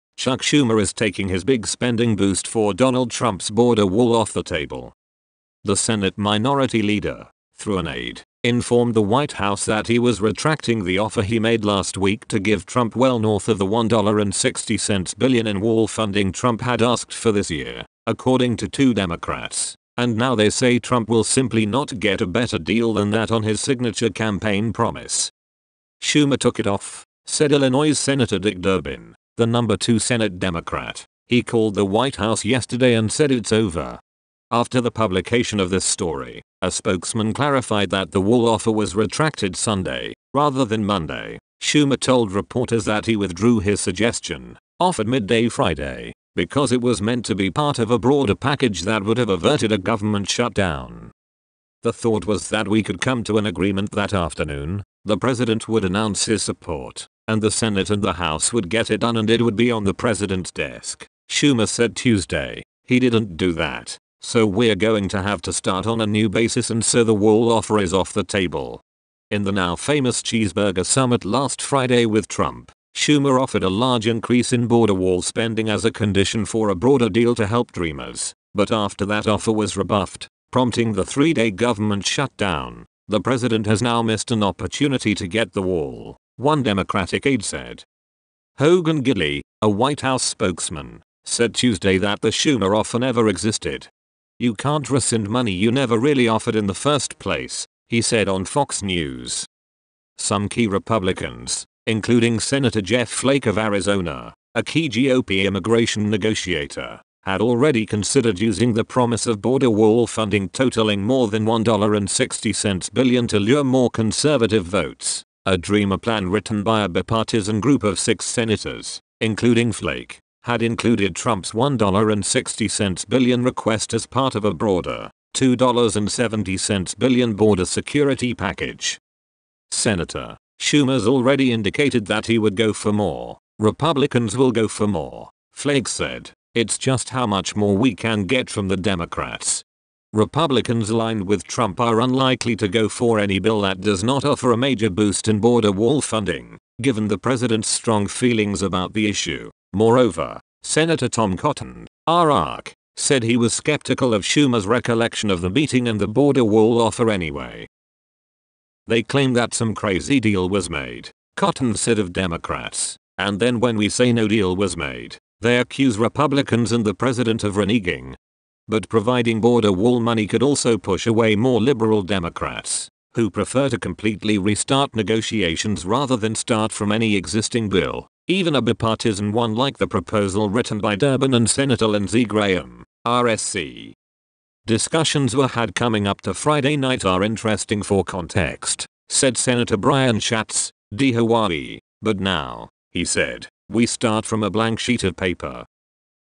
Schumer withdraws offer on Trump's wall
Republicans aligned with Trump are unlikely to go for any bill that does not offer a major boost in border wall funding.